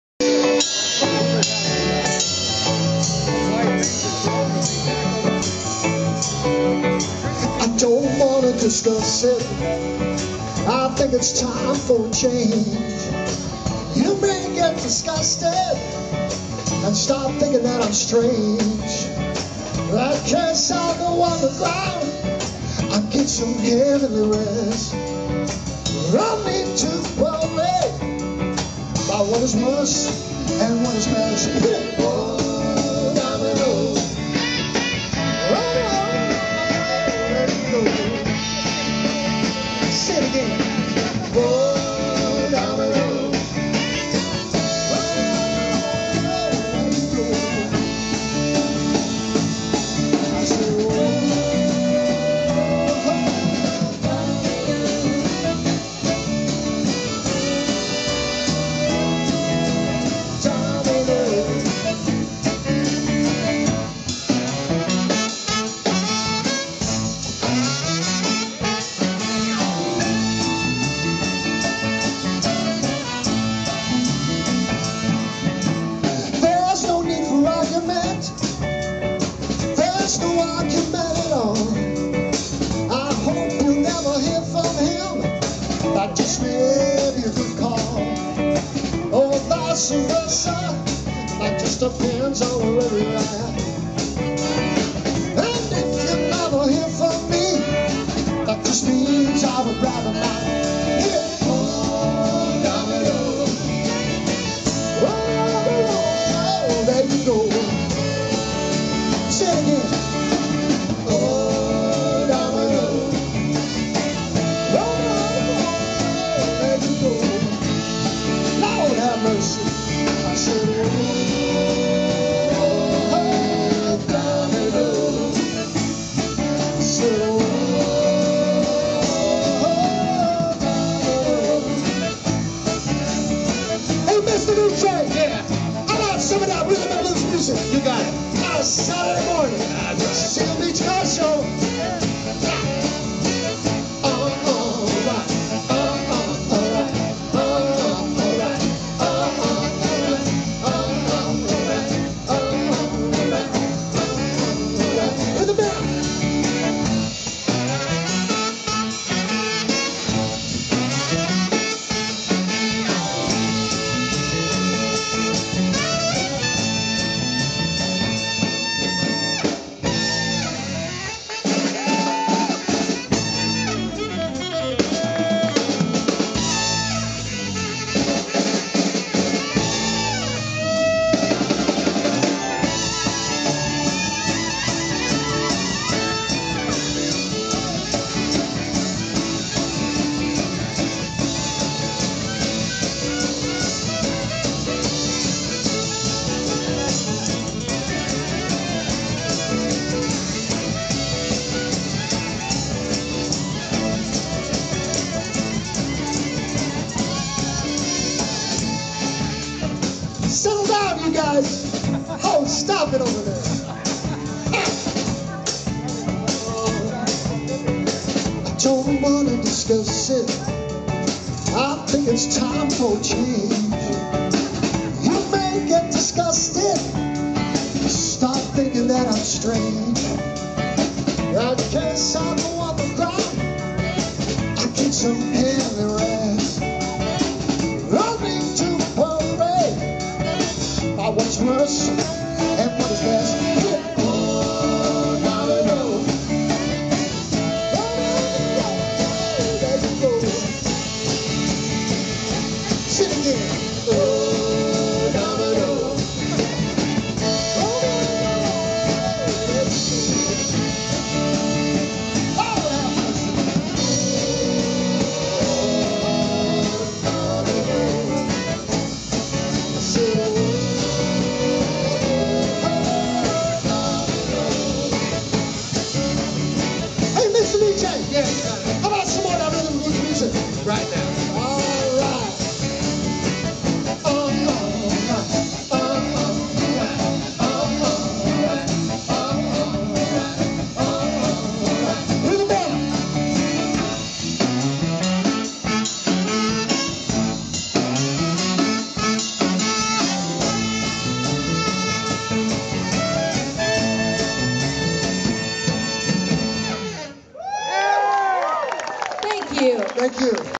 Sugah Daddy Band 03
Sugah Daddy Band @ the Seal Beach Car Show, Seal Beach California 04/28/2012